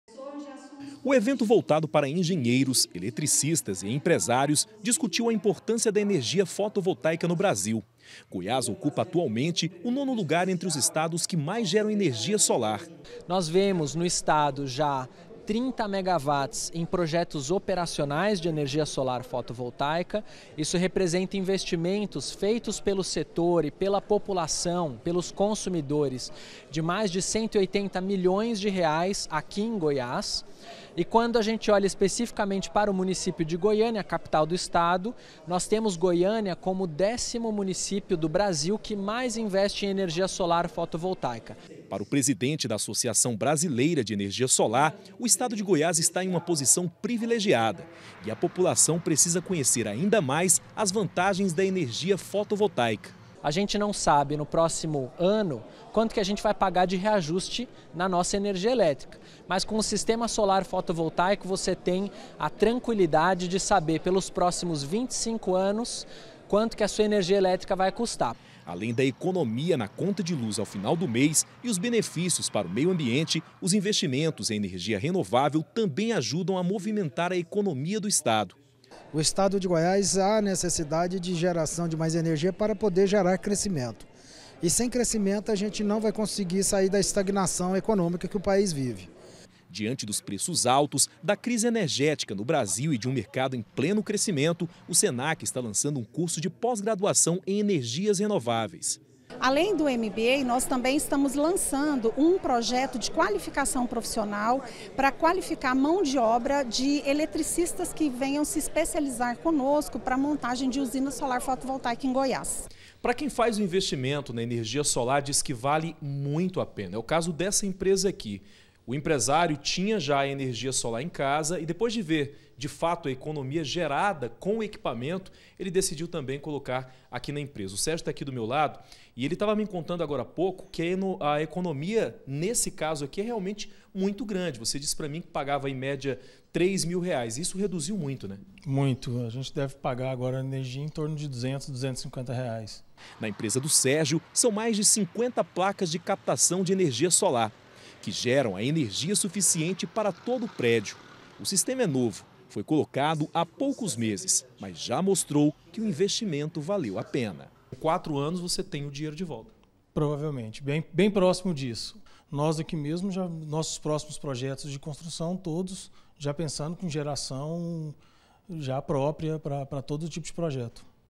ENERGIA SOLAR: SISTEMA GERA ECONOMIA NA CONTA DE LUZ
A energia solar ganha cada dia mais espaço. Goiás é um dos estados que mais investem em energia solar. O negócio parece promissor. Tanto que o SENAC lançou hoje um curso de pós-graduação em energias renováveis. O que inclui energia solar.